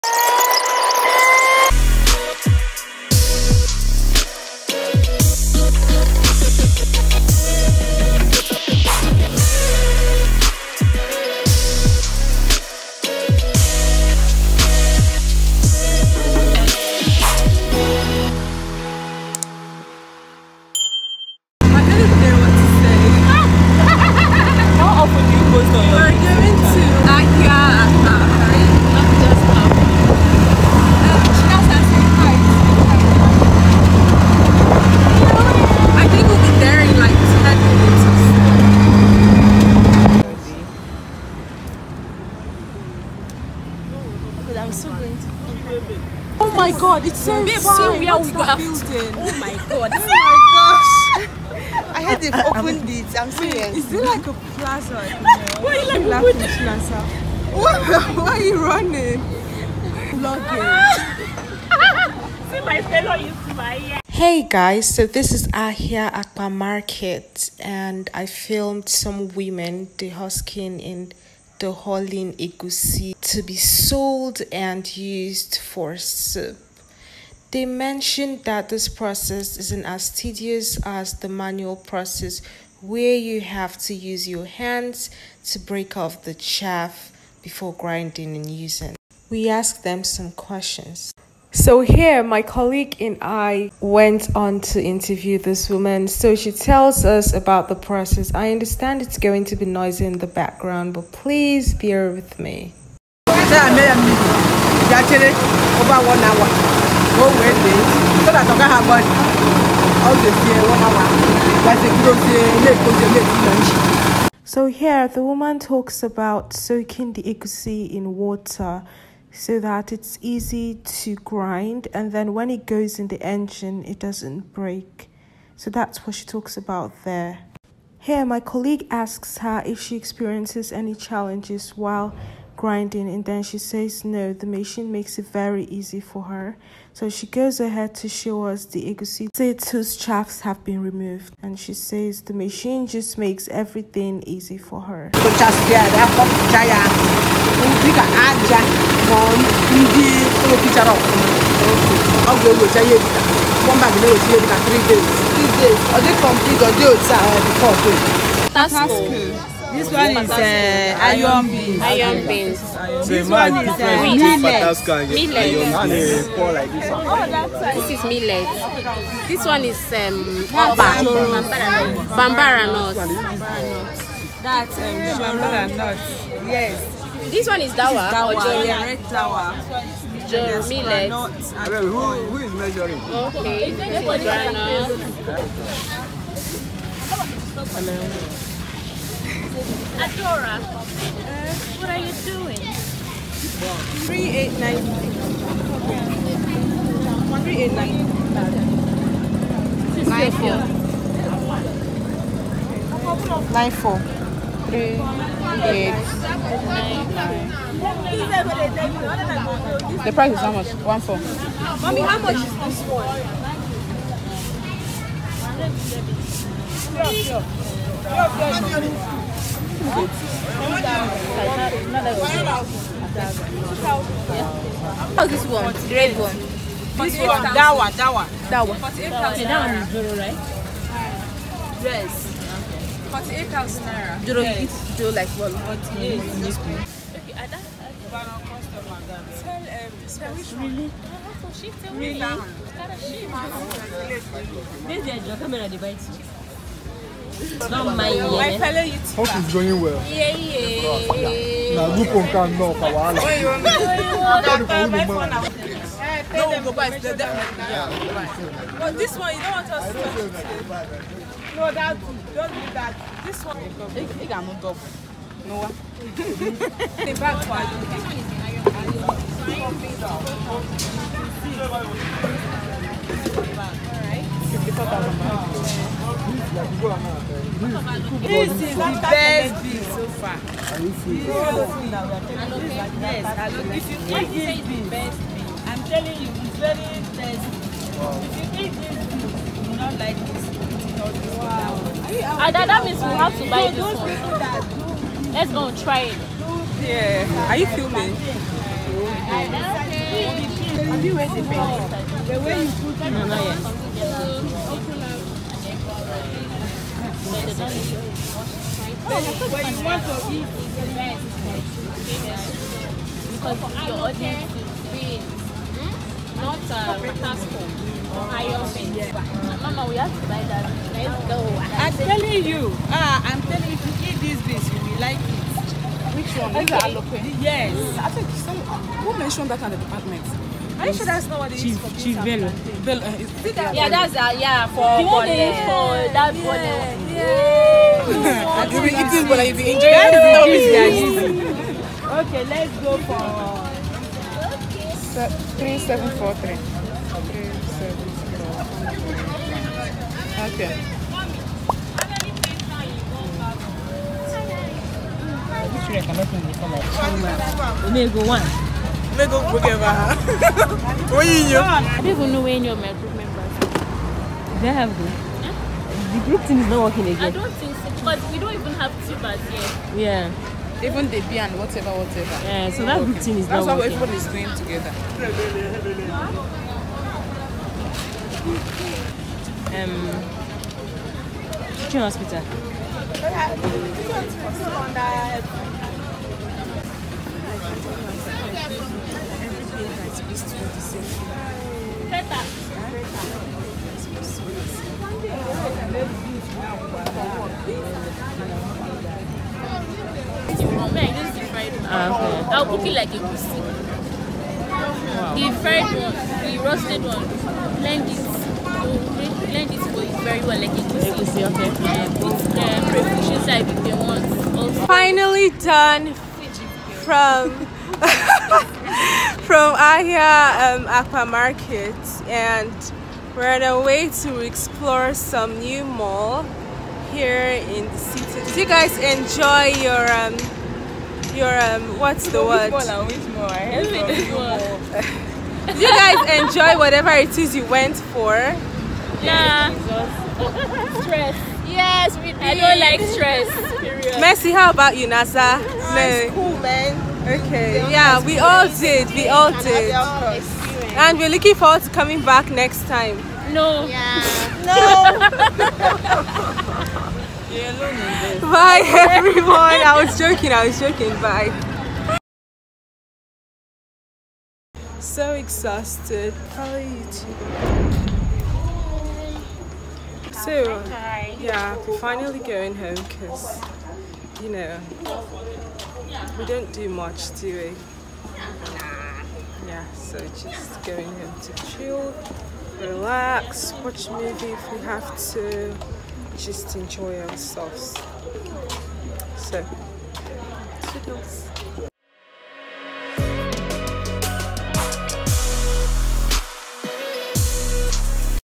MarketVlog At a Popular Market In Ebonyi State, Nigeria | Ada Unleashed
Hey guys! In today’s video. I vlogged our trip (my friends and I) to a local Nigerian market. We were sent by our supervisors to carry out a market survey at various markets. My group members and I were assigned to visit “AHIA AKPA MARKET” in EBONYI STATE, NIGERIA.

-Make sure you watch this video all through the end.
-Leave a comment in the comment section.

I genuinely appreciate all the love and support I constantly get from my subscribers.

I love y’all! We remain one family.☺️☺️